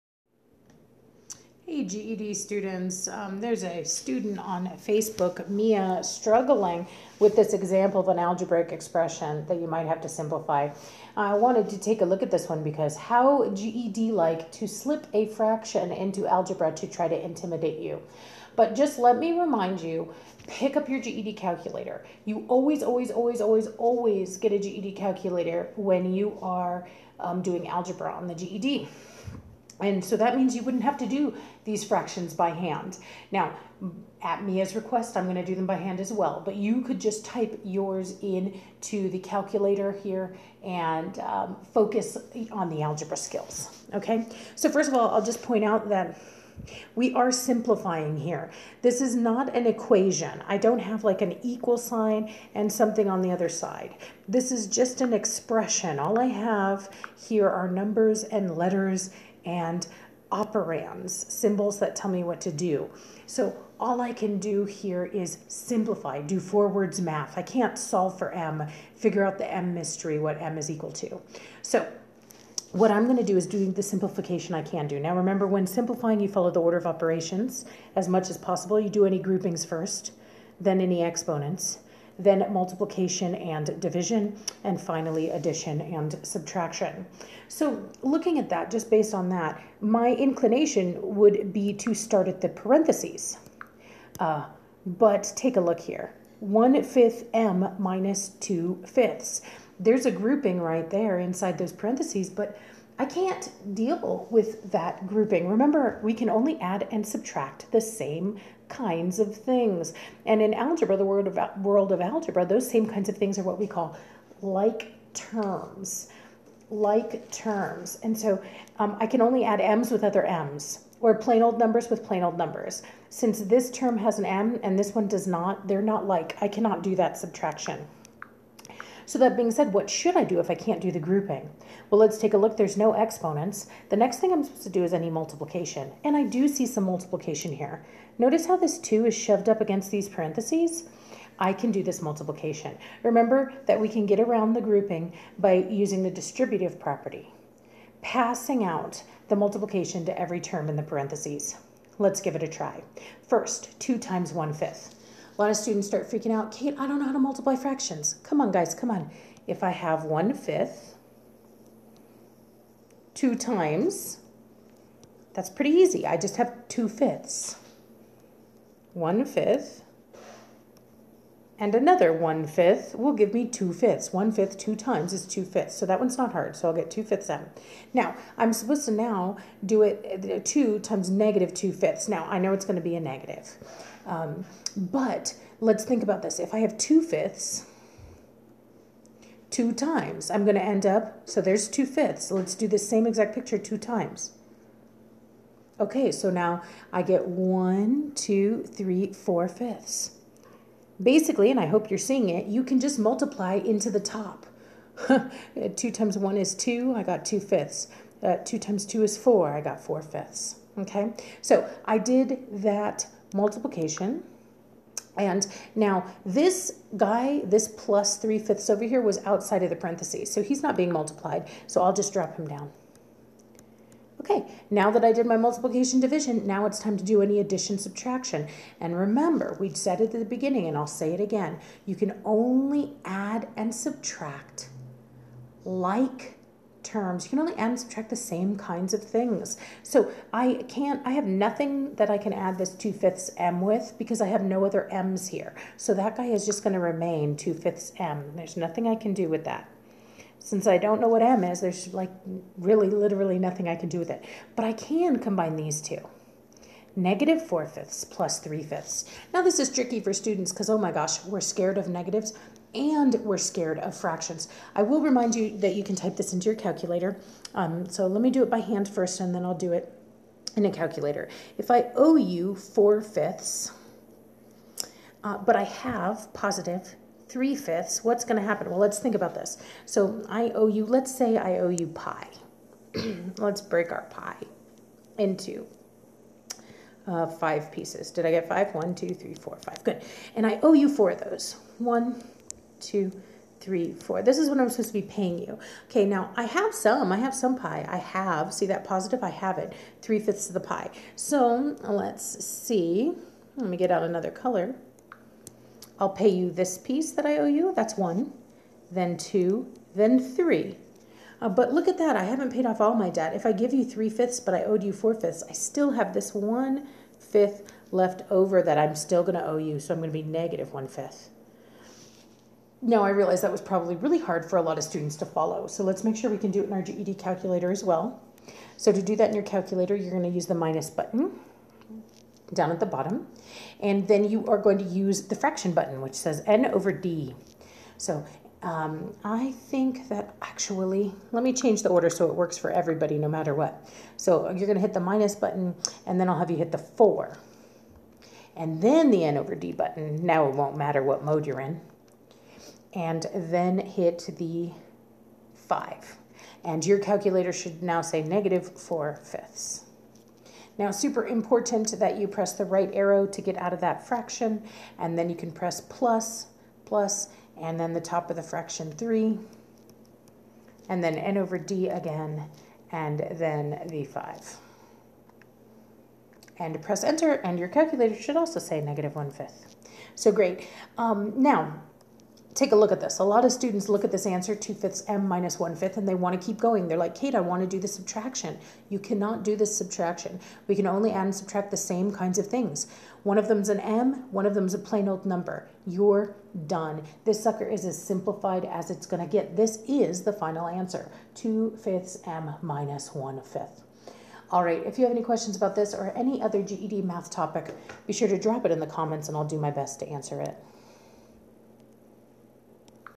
GED® Math: Simplify Expression with Fractions & Distribution (1.8, Adv, # 13)
Algebra Essentials, Multiplication with Distribution, Advanced Level, # 13

Order the Algebra Essentials Student Workbook, A Personalized Path Learning Adventure from Light & Salt Learning

GED Student Mia posted a question about this tricky algebraic expression full of fractions. Follow along as Kate uses the order of operations, distribution and combining like terms to simplify this expression.